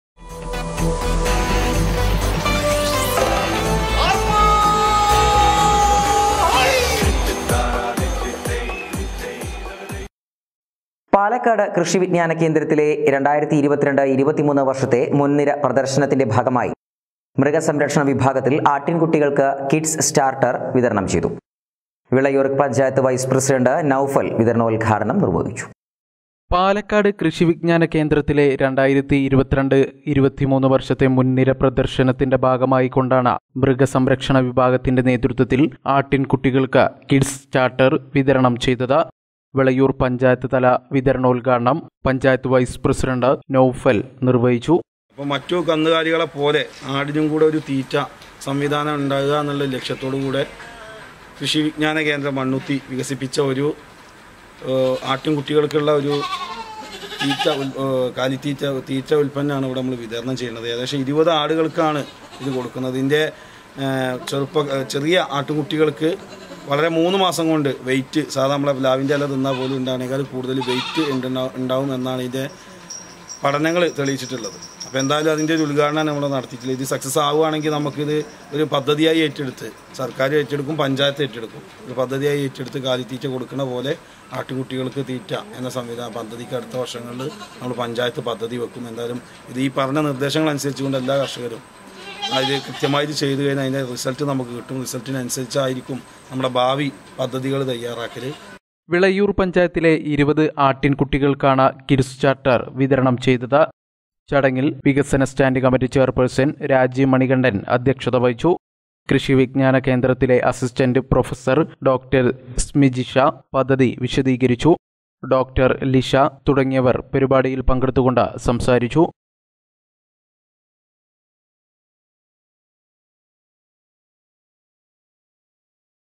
വിളയൂർ പഞ്ചായത്തിൽ ആട്ടിൻ കുട്ടികൾക്ക് കിഡ് സ്റ്റാർട്ടർ വിതരണം ചെയ്തു
വിളയൂർ പഞ്ചായത്തിൽ മൃഗസംരക്ഷണ വിഭാഗത്തിൽ ആട്ടിൻ കുട്ടികൾക്ക് കിഡ് സ്റ്റാർട്ടർ വിതരണം ചെയ്തു